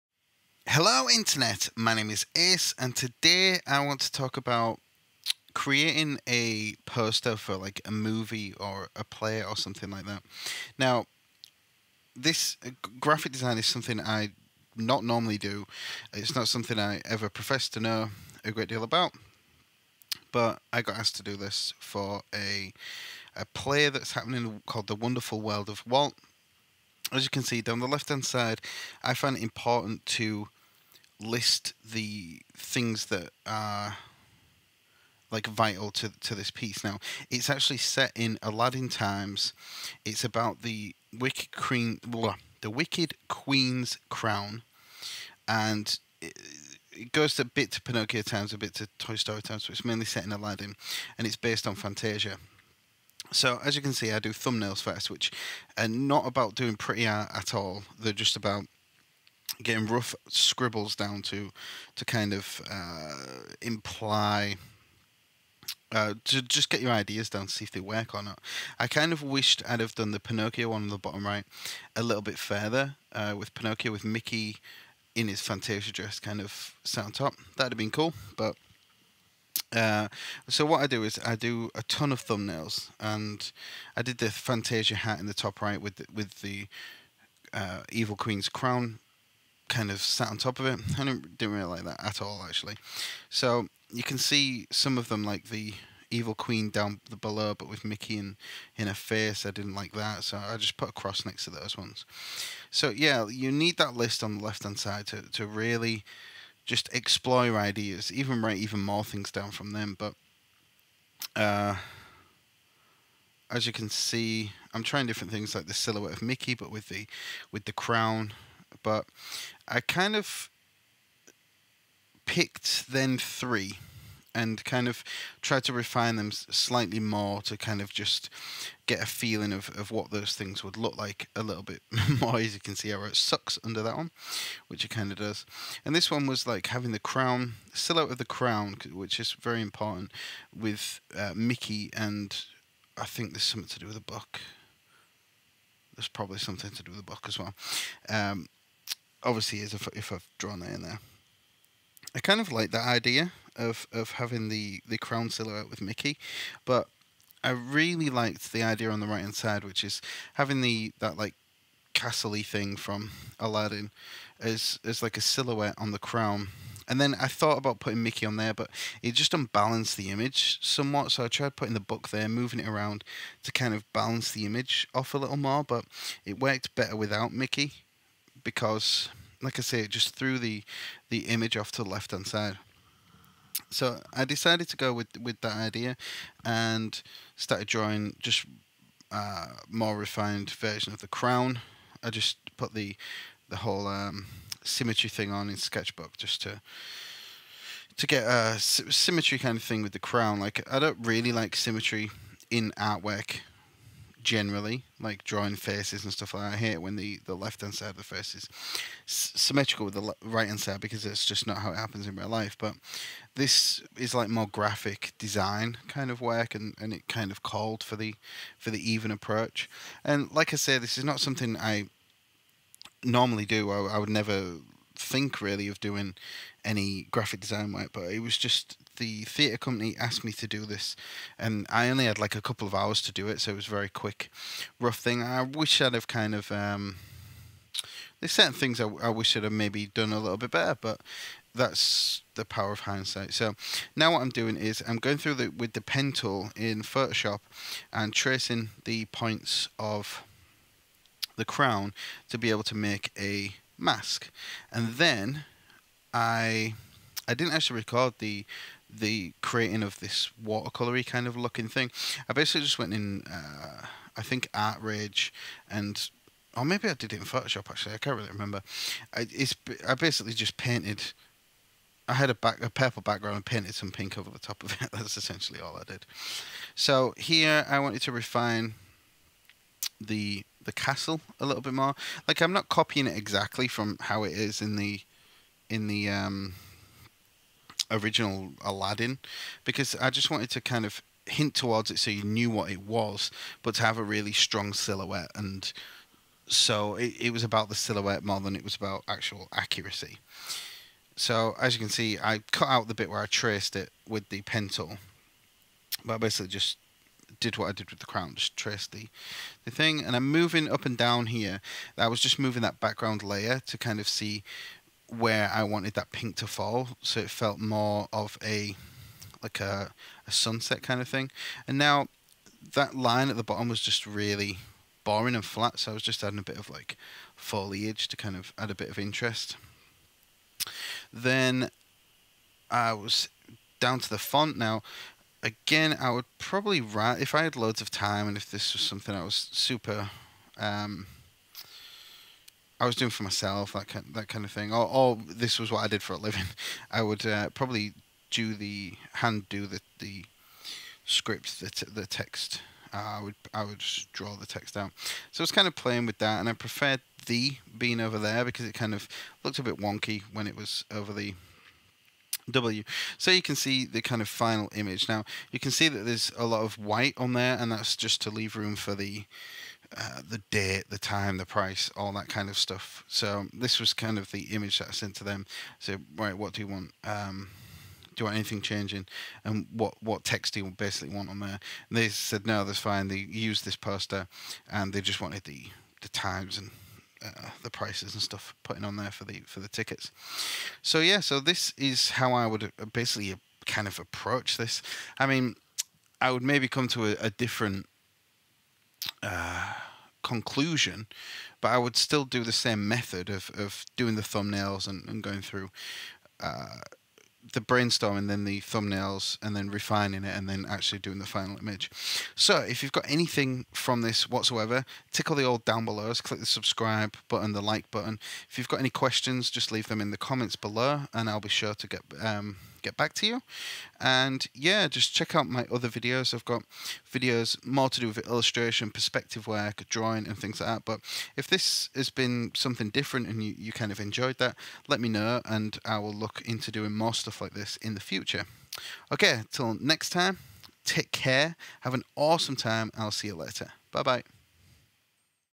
How to Design a Movie Poster
Hello internet! I'm here today to show you how I created a poster for a play, but the same process can be applied to movie posters, book covers or anything else really.

I'm not usually a graphic designer, but the same illustration processes can be applied when you design a movie poster.